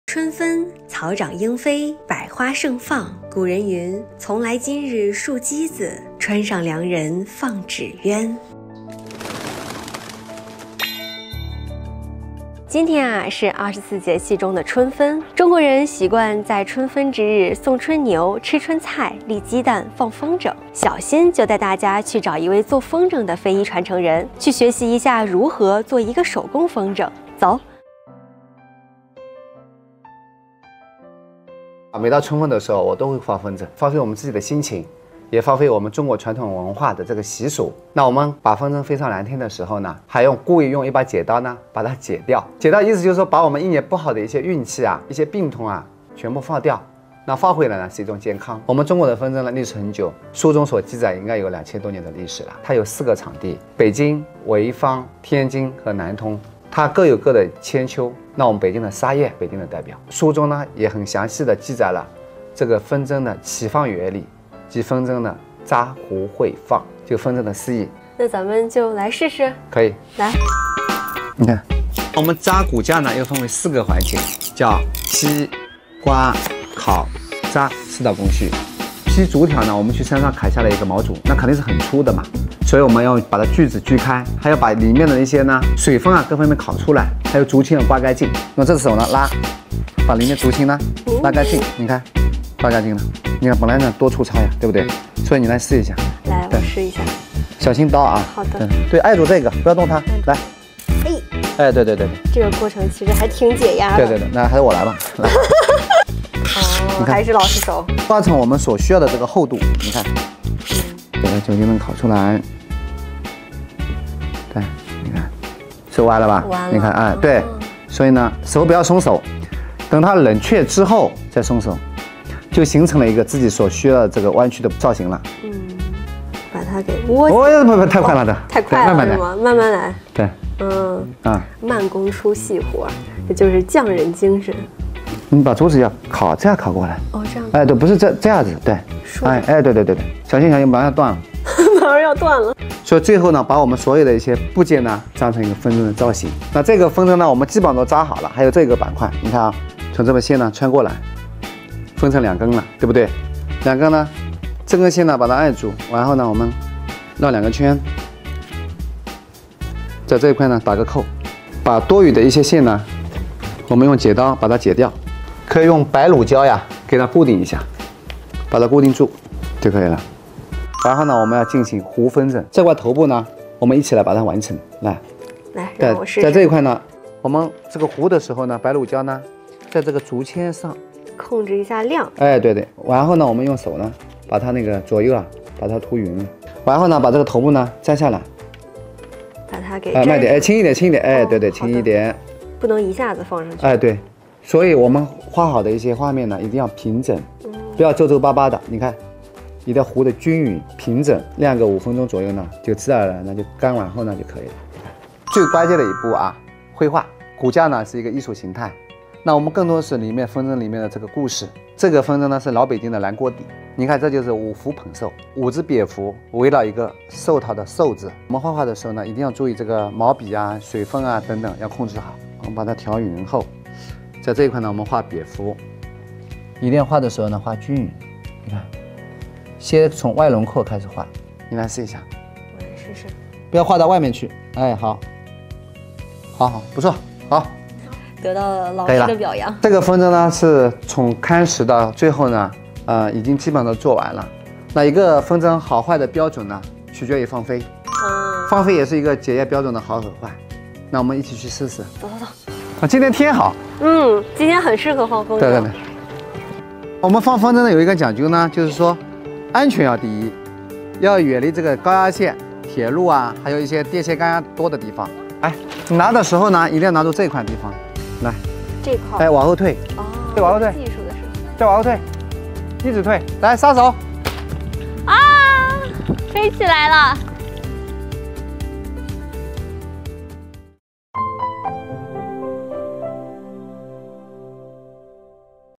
迎春分放纸鸢！小新带你沉浸式体验制作风筝
春分时节，草长莺飞，百花盛放。古人云：“从来今日竖鸡子，川上良人放纸鸢。”中国人习惯在春分之日送春牛、吃春菜、立鸡蛋、放风筝。那么今天，小新就带大家去找一位做风筝的非遗传承人，体验如何制作手工风筝。（李佳悦 董语飞）